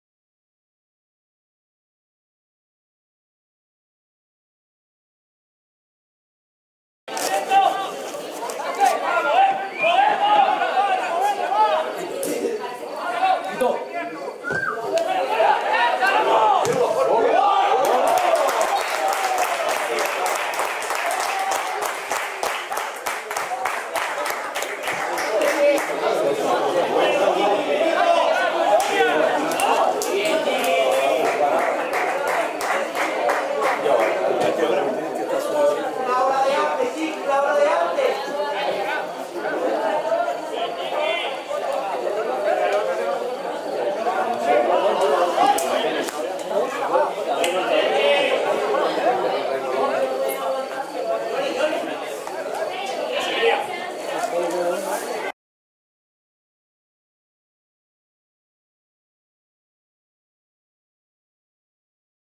Gol de Chiqui, jugador de la UD Teror 6 11 2015
Gol de Chiqui, jugador de la UD Teror, minuto 29 (segunda parte) 3-1. Partido de fútbol UD Teror-Hespérides CF de Categoría Regional Preferente Las Palmas. Temporada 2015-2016. Jornada 10. 6/11/2015. Campo de Fútbol Municipal de El Pino. Teror. Resultado final: 3-2